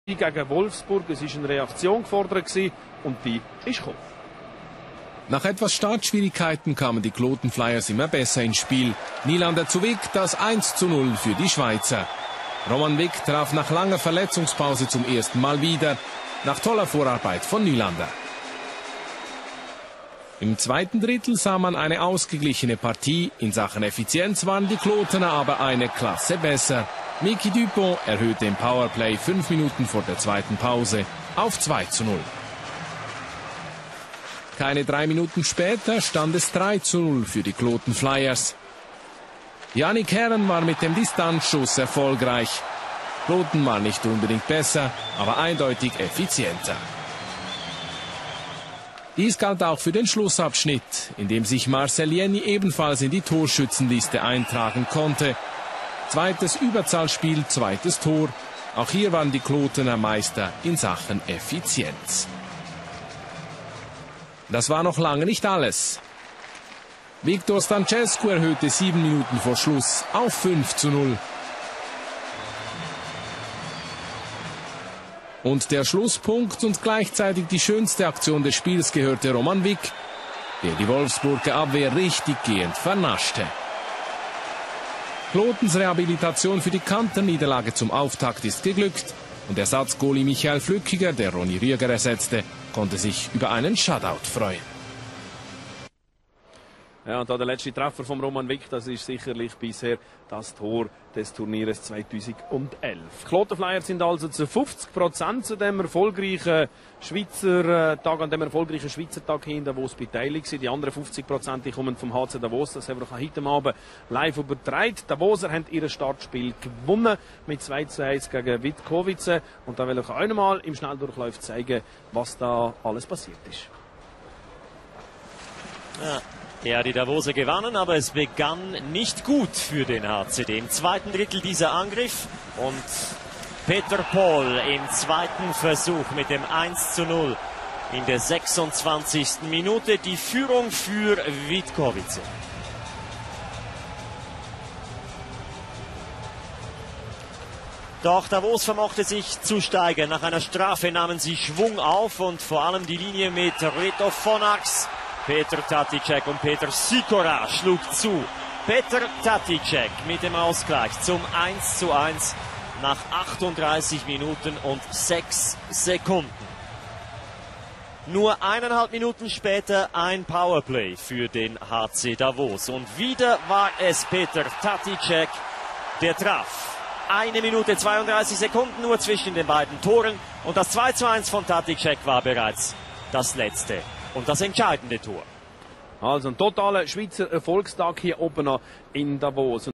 SF: Spengler Cup Spieltag 2
highlights from
Kloten Flyers - EHC Wolfsburg 6:0
HC Davos - HC vitkovice steel  2:1